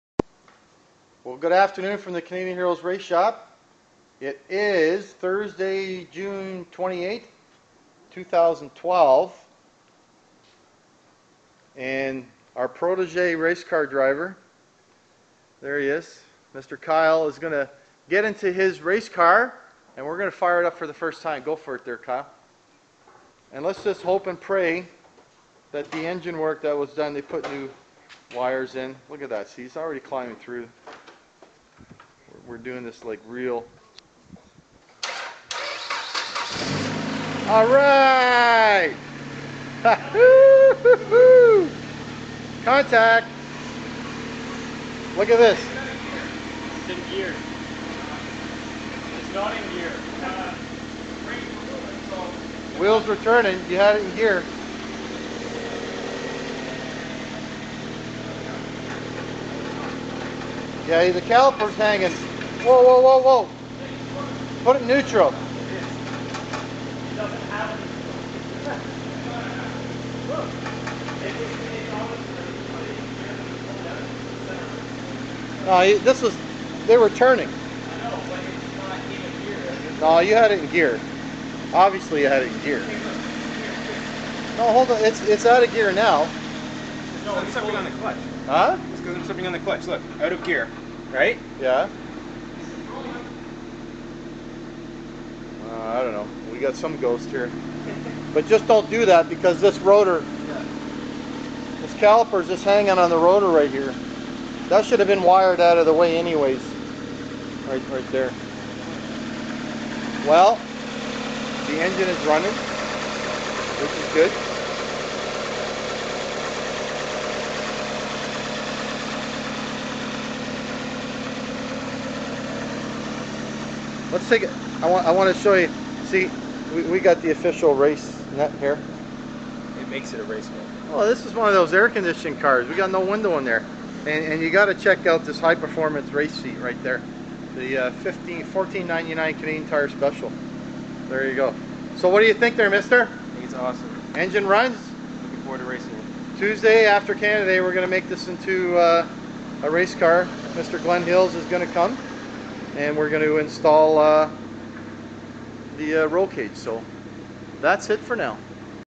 Canadian Heroes Pure Stock Nissan
Kyle and Jesse are working away on the Pure Stock, and this is the first fire up after giving her a tune up!